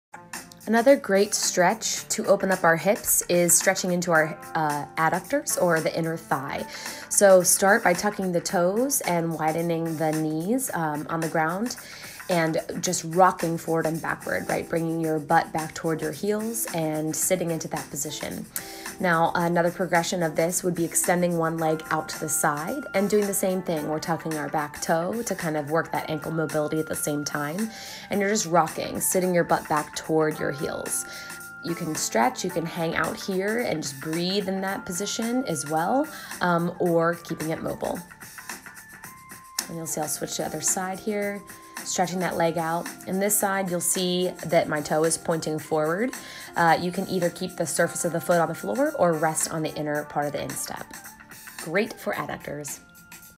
Tactical Frog / Adductor Rockback
Promotes mobility in the adductor and groin/inner thigh.